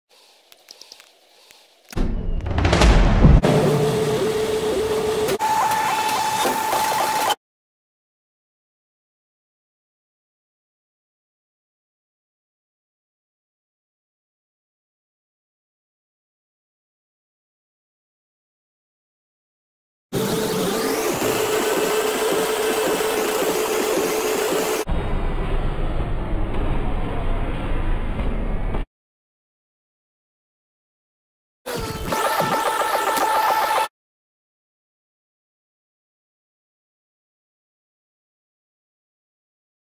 THIS WOULD'VE BEEN THE GREATEST POWER PLANT RUN EVER :(
I used the speed drink, donut, volt bike, and everything, but still...